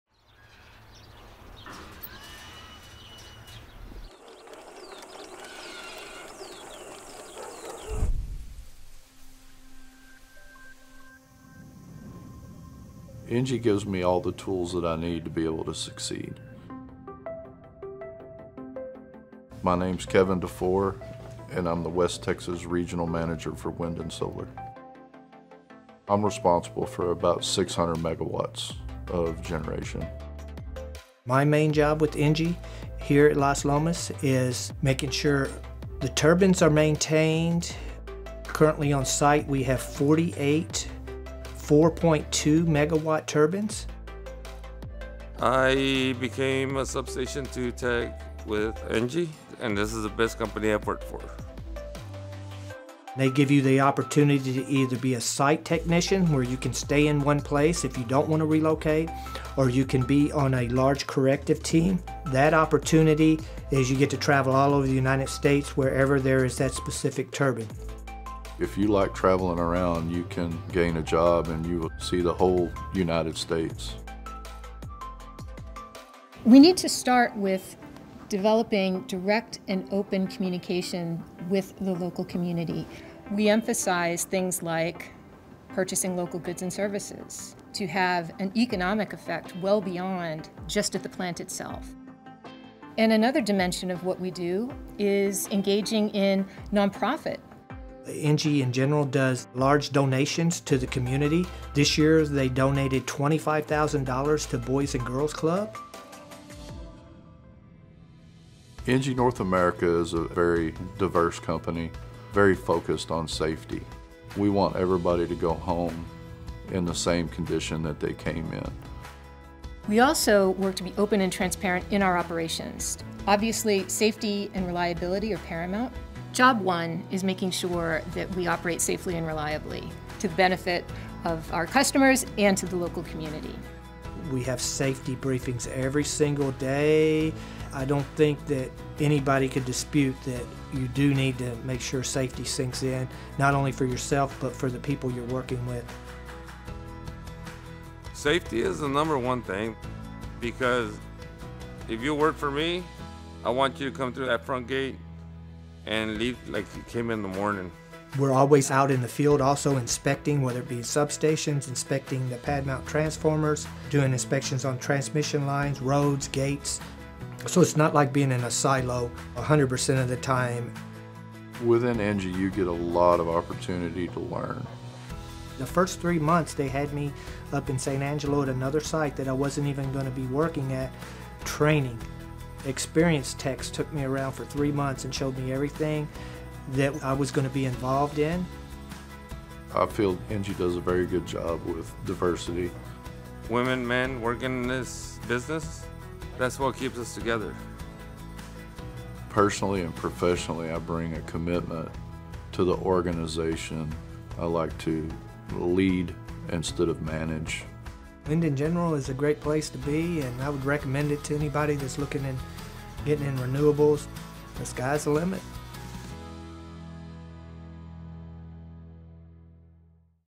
Las Lomas Wind Farm - Meet the ENGIE Team Helping to Harness the Wind in Zapata, Texas
"It takes a village" - In this case it takes a diverse team of individuals to operate and maintain the 48 turbines at ENGIE's Las Lomas Wind Farm in Zapata, Texas. Each turbine can generate 4.2MW of renewable power, supporting America's energy transition and bringing together a skilled and dedicated workforce that not only keeps things running safely and efficiently, but is also proudly connected to our neighbors and community in this part of South Texas.

Why not join us in our energy transition journey at our operations and developments all across the US - Come visit us